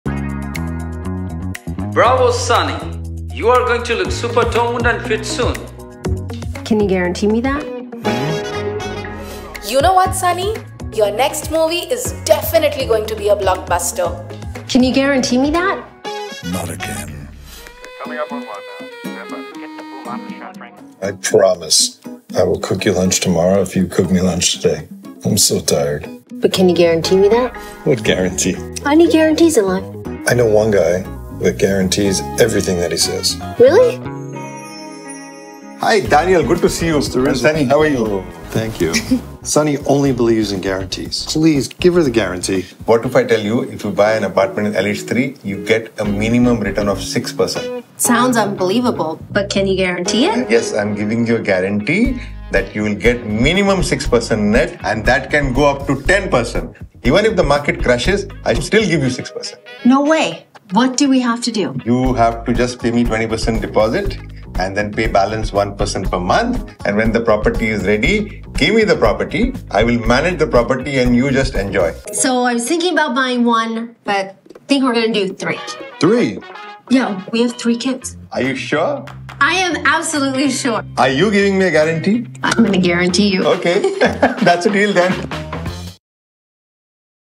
Danube Properties TVC featuring Sunny Leone
When it comes to Guarantee, even Sunny Leone knows that Danube Properties delivers.

Danube Properties commercial featuring Sunny Leone.

A MASTERKOPY FILMS production, 2023